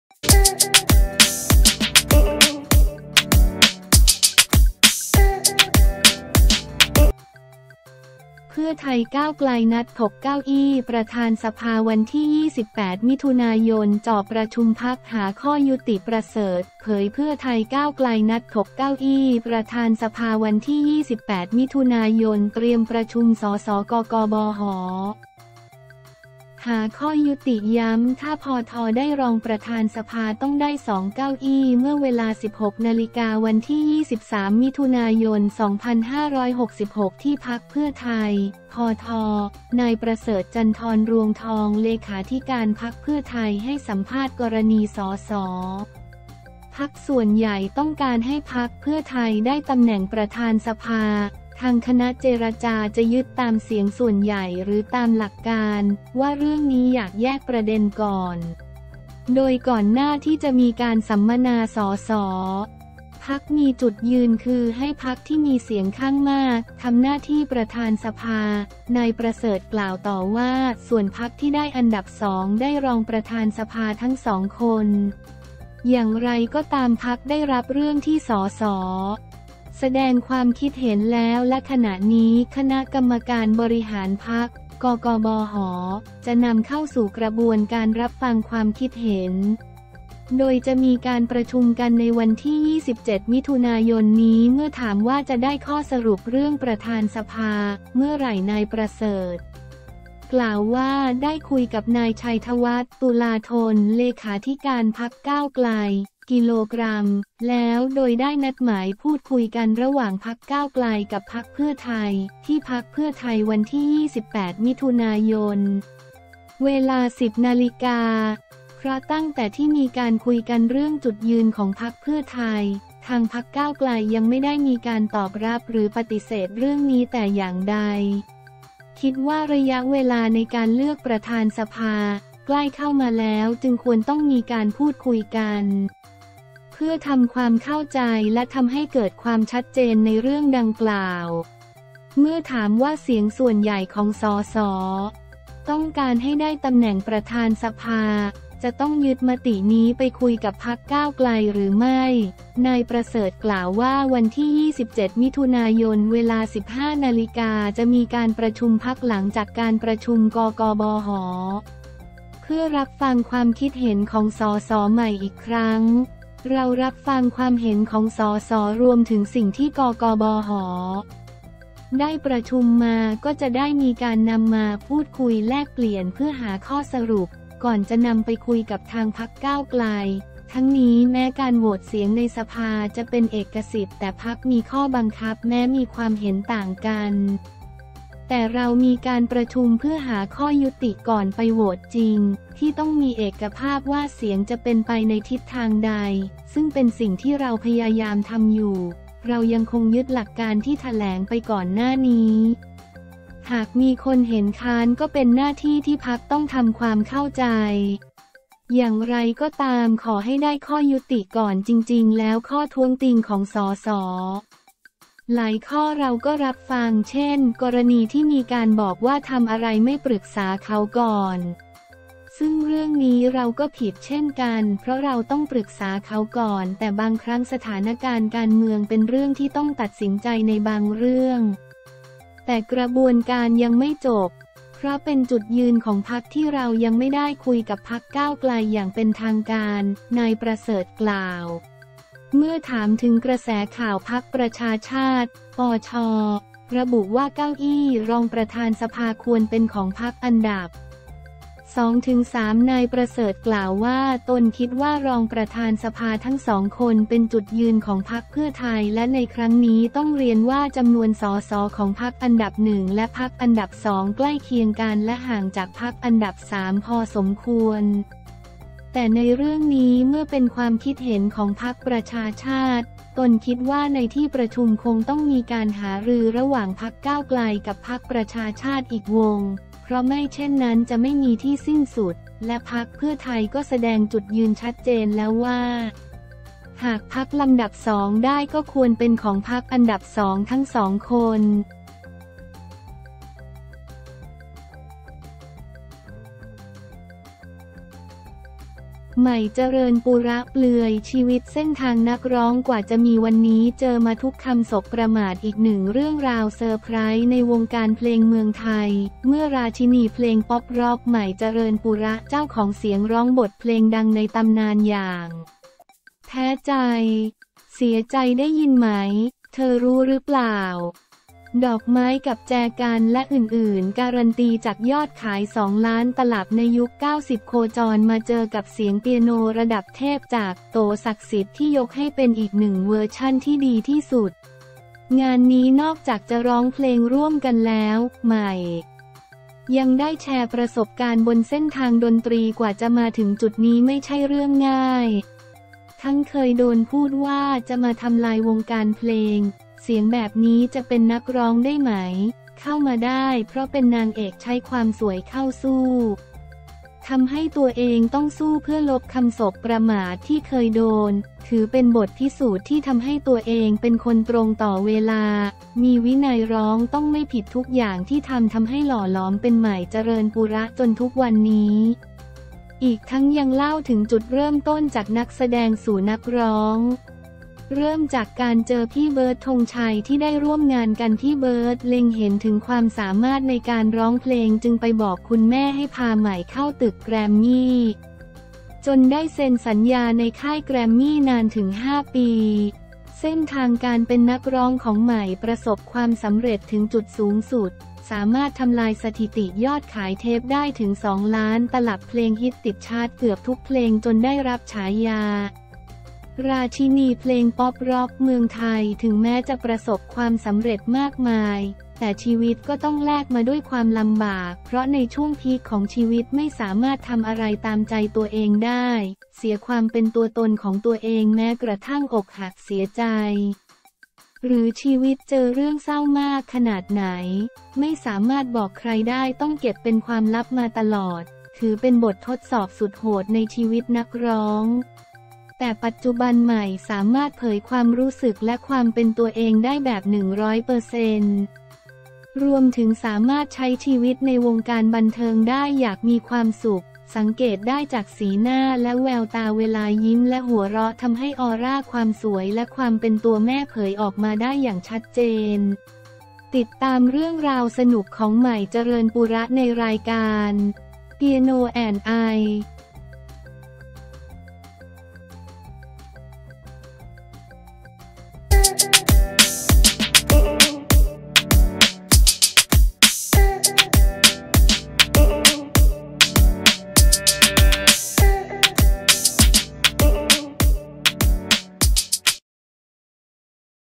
เพื่อไทย ก้าวไกล นัดถกเก้าอี้ประธานสภา 28 มิ ย จ่อประชุมพรรค หาข้อยุติ
ประเสริฐ” เผย เพื่อไทย-ก้าวไกล นัดถกเก้าอี้ประธานสภาฯ 28 มิ.ย. เตรียมประชุม ส.ส.-กก.บห. หาข้อยุติ ย้ำ ถ้าพท.ได้รองประธานสภาฯ ต้องได้ 2 เก้าอี้

เมื่อเวลา 16.00 น. วันที่ 23 มิ.ย. 2566 ที่พรรคเพื่อไทย (พท.) นายประเสริฐ จันทรรวงทอง เลขาธิการพรรคเพื่อไทย ให้สัมภาษณ์กรณี ส.ส. พรรคส่วนใหญ่ต้องการให้พรรคเพื่อไทยได้ตำแหน่งประธานสภาฯ ทางคณะเจรจาจะยึดตามเสียงส่วนใหญ่หรือตามหลักการ ว่า เรื่องนี้อยากแยกประเด็นก่อน โดยก่อนหน้าที่จะมีการสัมมนา ส.ส. พรรคมีจุดยืนคือให้พรรคที่มีเสียงข้างมากทำหน้าที่ประธานสภาฯ
นายประเสริฐ กล่าวต่อว่า ส่วนพรรคที่ได้อันดับสอง ได้รองประธานสภาฯ ทั้ง 2 คน อย่างไรก็ตาม พรรคได้รับเรื่องที่ ส.ส. แสดงความคิดเห็นแล้ว และขณะนี้คณะกรรมการบริหารพรรค (กก.บห.) จะนำเข้าสู่กระบวนการรับฟังความคิดเห็น โดยจะมีการประชุมกันในวันที่ 27 มิ.ย.นี้

เมื่อถามว่าจะได้ข้อสรุปเรื่องประธานสภาฯ เมื่อไหร่ นายประเสริฐ กล่าวว่า ได้คุยกับนายชัยธวัช ตุลาธน เลขาธิการพรรคก้าวไกล (ก.ก.) แล้ว โดยได้นัดหมายพูดคุยกันระหว่างพรรคก้าวไกลกับพรรคเพื่อไทย ที่พรรคเพื่อไทย วันที่ 28 มิ.ย. เวลา 10.00 น. เพราะตั้งแต่ที่มีการคุยกันเรื่องจุดยืนของพรรคเพื่อไทย ทางพรรคก้าวไกลยังไม่ได้มีการตอบรับหรือปฏิเสธเรื่องนี้แต่อย่างใด คิดว่าระยะเวลาในการเลือกประธานสภาฯ ใกล้เข้ามาแล้วจึงควรต้องมีการพูดคุยกัน เพื่อทำความเข้าใจและทำให้เกิดความชัดเจนในเรื่องดังกล่าว

เมื่อถามว่าเสียงส่วนใหญ่ของ ส.ส. ต้องการให้ได้ตำแหน่งประธานสภาฯ จะต้องยึดมตินี้ไปคุยกับพรรคก้าวไกลหรือไม่ นายประเสริฐ กล่าวว่า วันที่ 27 มิ.ย. เวลา 15.00 น. จะมีการประชุมพรรค หลังจากการประชุม กก.บห. เพื่อรับฟังความคิดเห็นของส.ส.ใหม่อีกครั้ง เรารับฟังความเห็นของส.ส. รวมถึงสิ่งที่ กก.บห. ได้ประชุมมาก็จะได้มีการนำมาพูดคุยแลกเปลี่ยนเพื่อหาข้อสรุป ก่อนจะนำไปคุยกับทางพรรคก้าวไกล
“ทั้งนี้ แม้การโหวตเสียงในสภาฯ จะเป็นเอกสิทธิ์ แต่พรรคมีข้อบังคับ แม้มีความเห็นต่างกัน แต่เรามีการประชุมเพื่อหาข้อยุติก่อนไปโหวตจริง ที่ต้องมีเอกภาพว่าเสียงจะเป็นไปในทิศทางใด ซึ่งเป็นสิ่งที่เราพยายามทำอยู่ เรายังคงยึดหลักการที่แถลงไปก่อนหน้านี้ หากมีคนเห็นค้านก็เป็นหน้าที่ที่พรรคต้องทำความเข้าใจ

อย่างไรก็ตาม ขอให้ได้ข้อยุติก่อน จริงๆ แล้วข้อท้วงติงของ ส.ส. หลายข้อเราก็รับฟัง เช่น กรณีที่มีการบอกว่าทำอะไรไม่ปรึกษาเขาก่อน ซึ่งเรื่องนี้เราก็ผิดเช่นกัน เพราะเราต้องปรึกษาเขาก่อน แต่บางครั้งสถานการณ์การเมือง เป็นเรื่องที่ต้องตัดสินใจในบางเรื่อง แต่กระบวนการยังไม่จบ เพราะเป็นจุดยืนของพรรคที่เรายังไม่ได้คุยกับพรรคก้าวไกลอย่างเป็นทางการ” นายประเสริฐ กล่าว

เมื่อถามถึงกระแสข่าวพรรคประชาชาติ (ปช.) ระบุว่าเก้าอี้รองประธานสภาฯ ควรเป็นของพรรคอันดับ 2-3 นายประเสริฐ กล่าวว่า ตนคิดว่ารองประธานสภาฯ ทั้ง 2 คน เป็นจุดยืนของพรรคเพื่อไทย และในครั้งนี้ต้องเรียนว่าจำนวน ส.ส.ของพรรคอันดับ 1 และพรรคอันดับ 2 ใกล้เคียงกัน และห่างจากพรรคอันดับ 3 พอสมควร แต่ในเรื่องนี้เมื่อเป็นความคิดเห็นของพรรคประชาชาติ ตนคิดว่าในที่ประชุมคงต้องมีการหารือระหว่างพรรคก้าวไกลกับพรรคประชาชาติอีกวง เพราะไม่เช่นนั้นจะไม่มีที่สิ้นสุด และพรรคเพื่อไทยก็แสดงจุดยืนชัดเจนแล้วว่า หากพรรคลำดับ 2 ได้ก็ควรเป็นของพรรคอันดับ 2 ทั้ง 2 คน